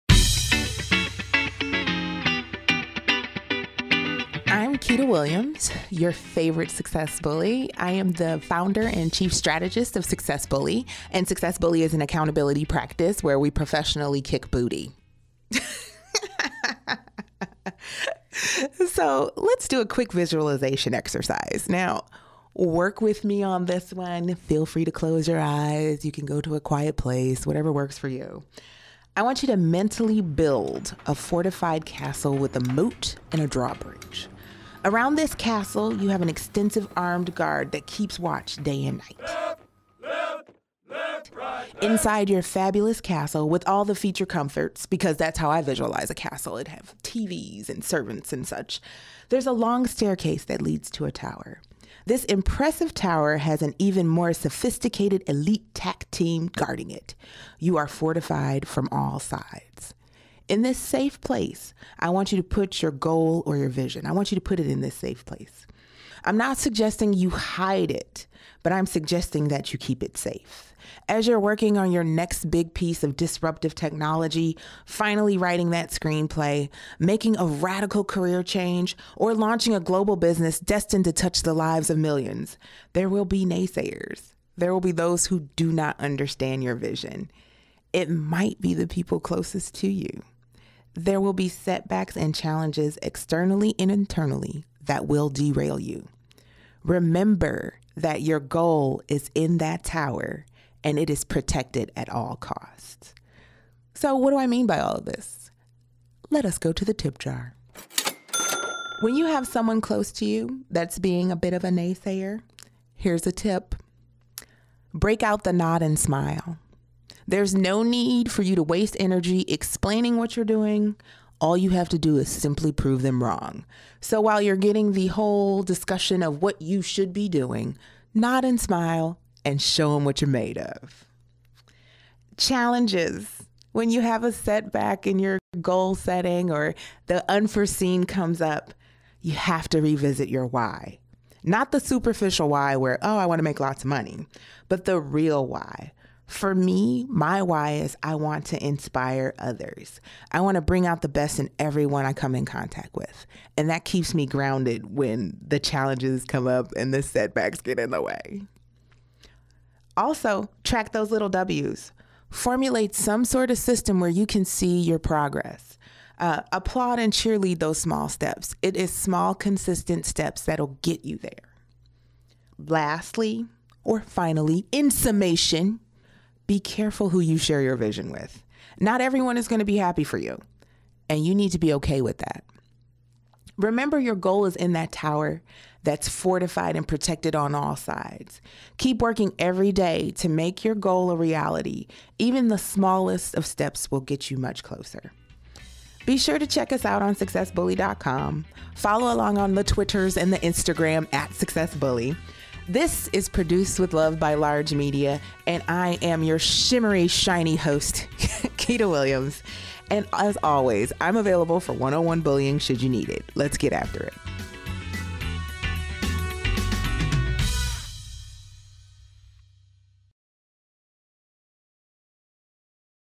Episode 10: Be on your Guard...
On Episode 10 of the Success Bully Podcast, I’m putting you on high alert. I’m asking you to “Be on Your Guard.” I want you to put your goal/vision in a fortified castle with a moat and drawbridge.&nbsp; Around this castle, you have an extensive armed guard the keeps watch day and night. As you’re working on the next big disruptive piece of technology, finally writing your screenplay, making a radical career change, launching a global business destined to touch the lives of millions, there will be naysayers who will not understand your vision. It's up to you to protect your vision!

  &nbsp;